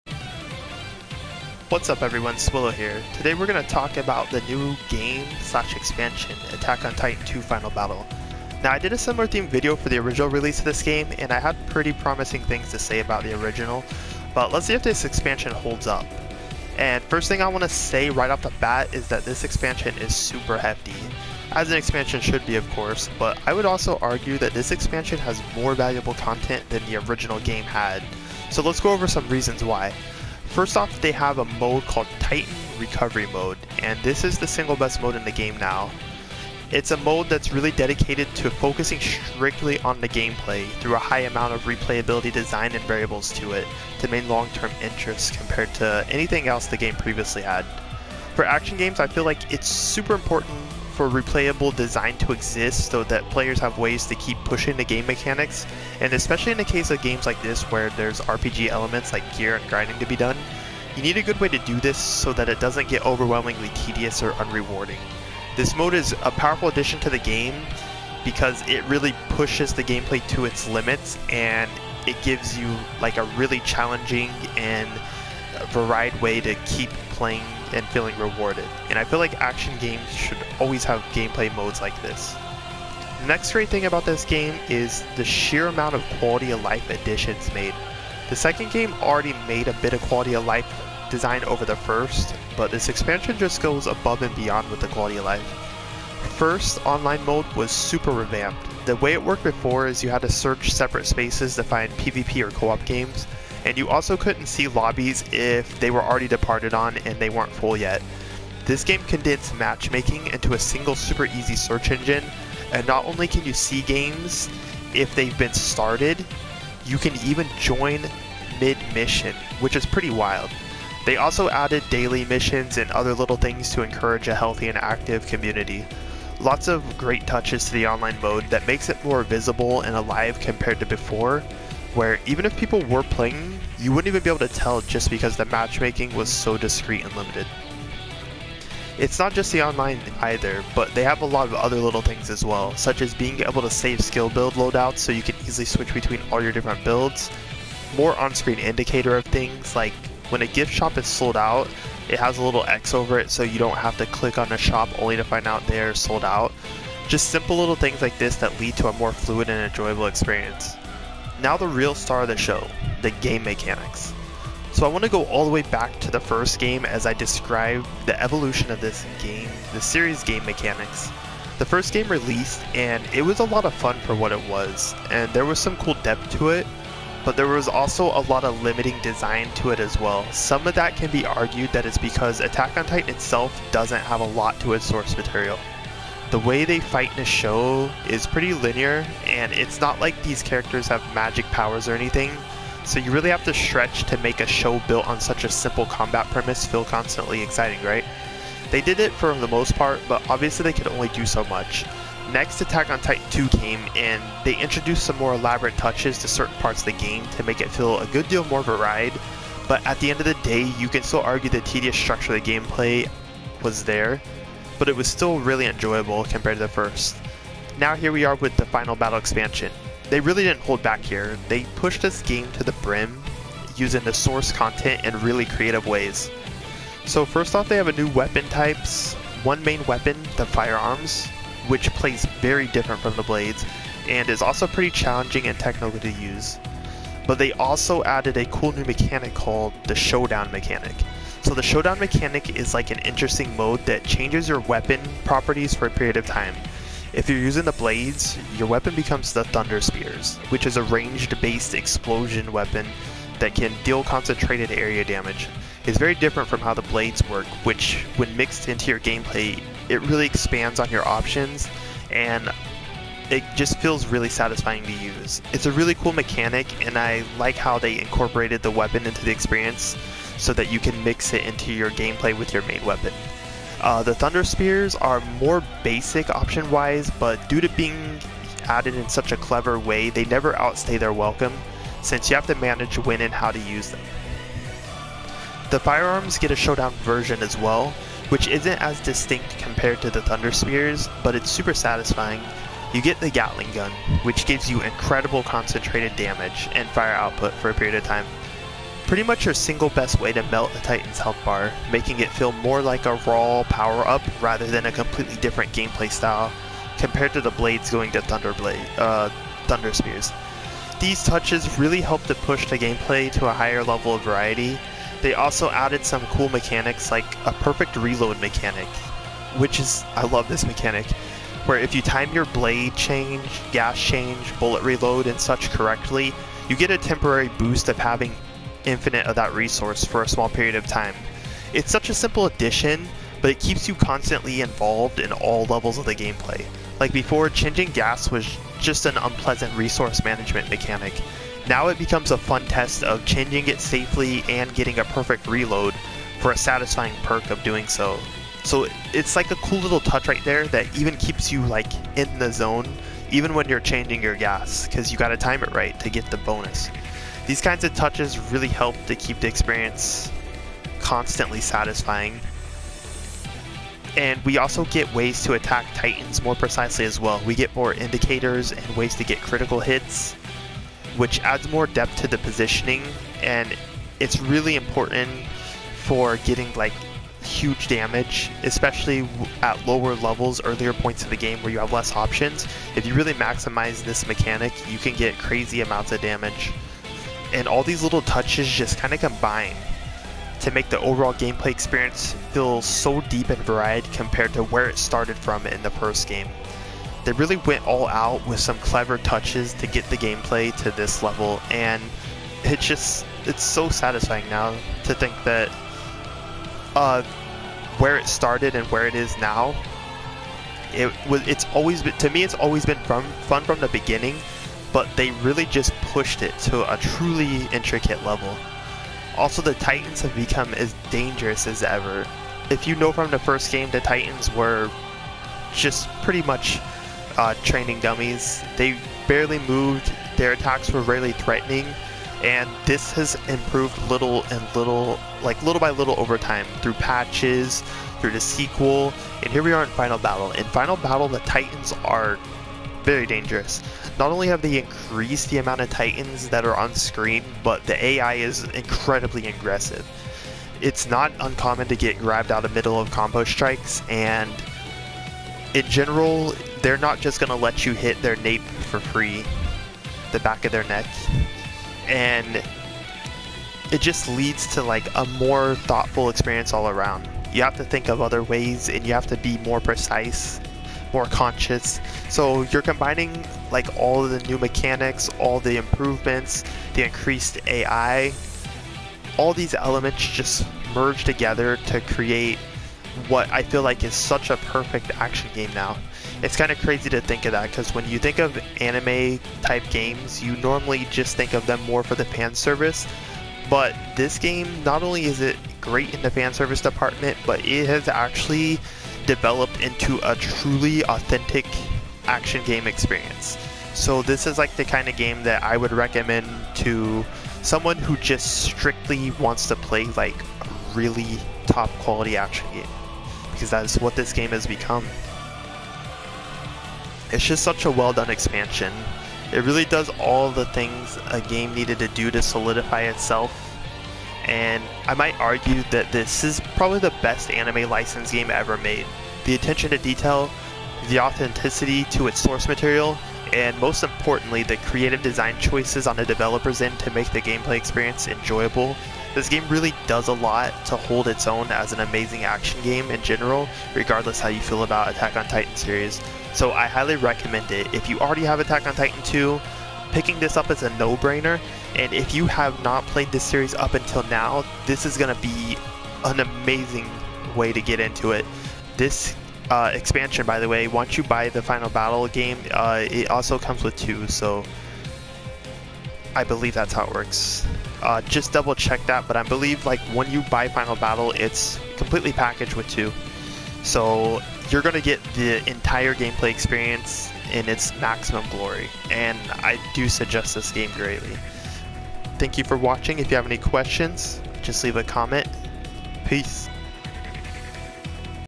Attack on Titan 2: Final Battle - Is it worth it?
I think many of you already know how I feel about this game, but here we go! Also let me know what you think about the game if you've been playing it yourself.

Also apparently I don’t know how to edit videos anymore. Not sure why it boxed the video so small, but uhh I guess that’s not the important part here. Sorry about that and I’ll have it fixed for future stuff.